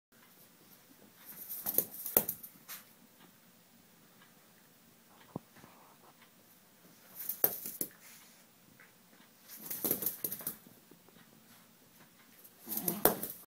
Zeb the Hungarian Puli....then there was the drunken fight!
Zeb the Hungarian Puli shaking something else like a polaroid picture!